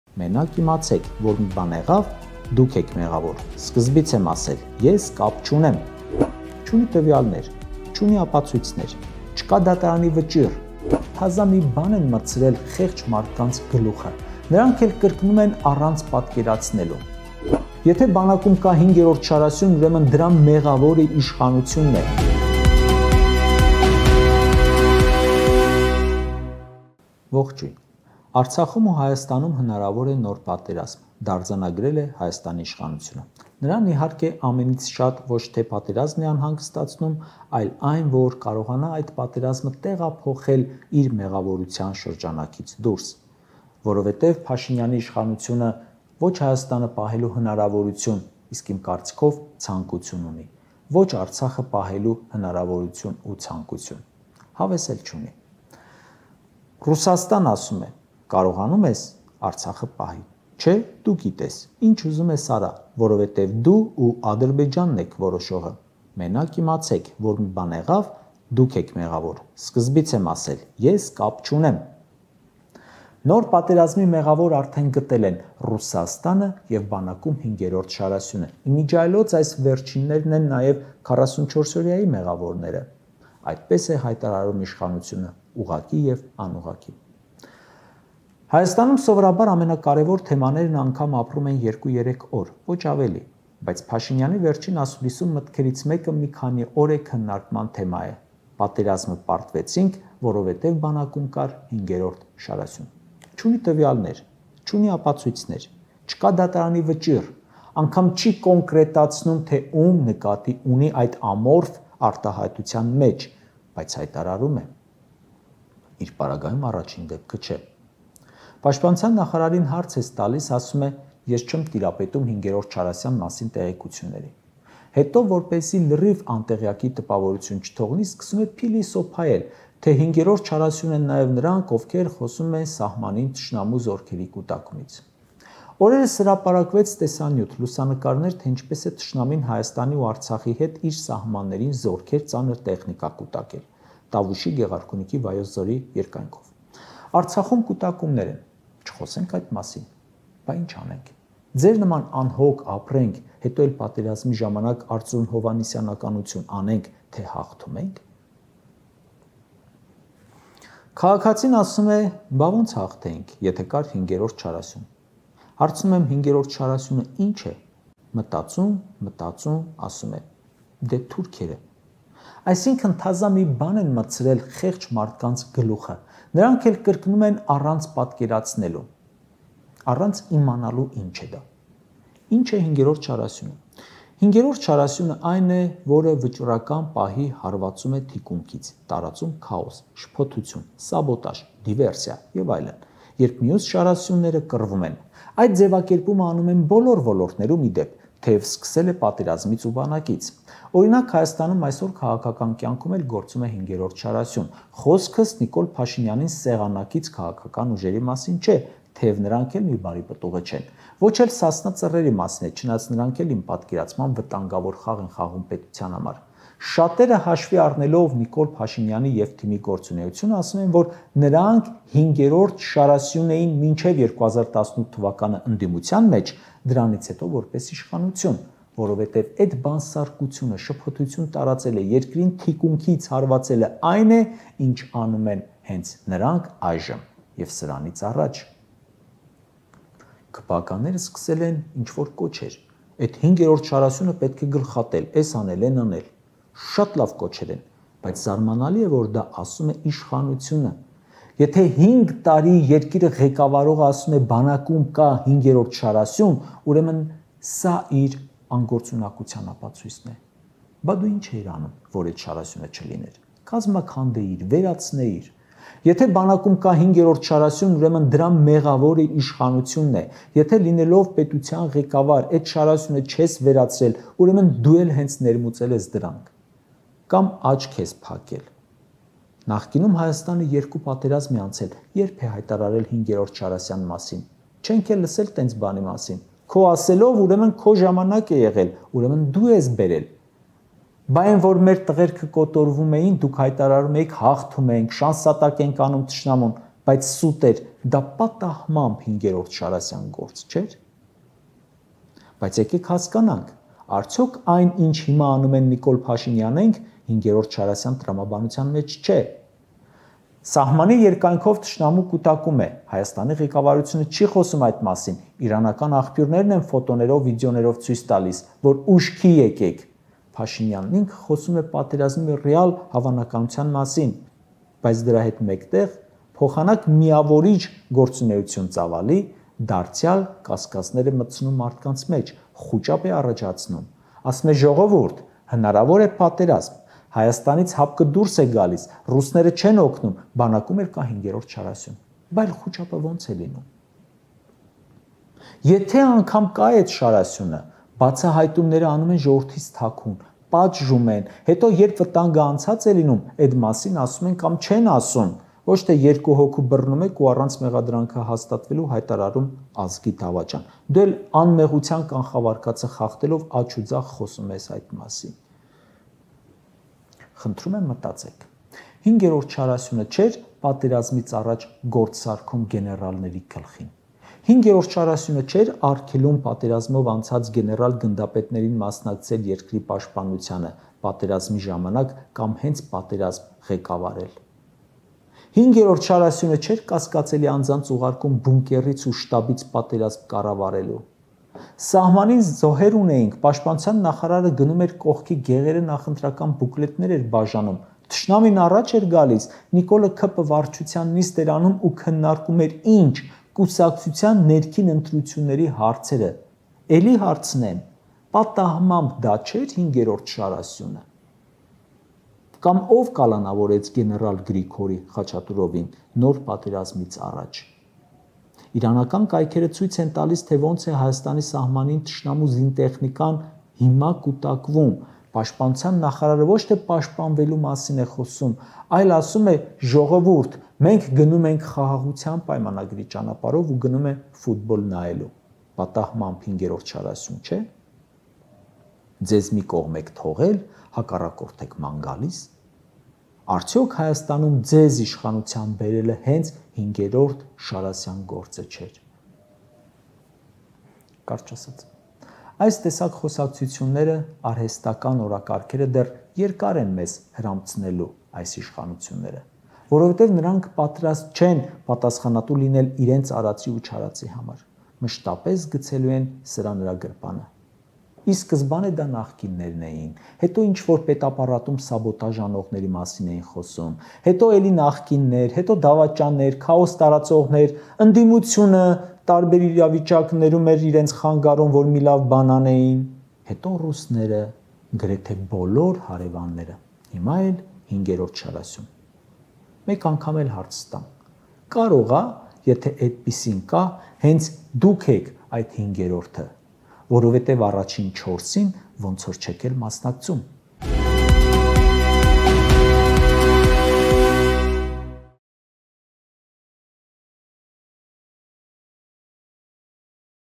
«Կարճ ասած»․ 5-րդ շարասյունը․ փաստեր, որոնցից խուսափել չի լինի
Հայտարարում են որ պատերազմը մոտ է, պաշտպանության նախարարը ֆուտբոլ է դիտում, վարչապետը քննարկում է ընտրությունները, գեներալին կալանավորում են․ Իսկ ո՞վ է 5-րդ շարասյունը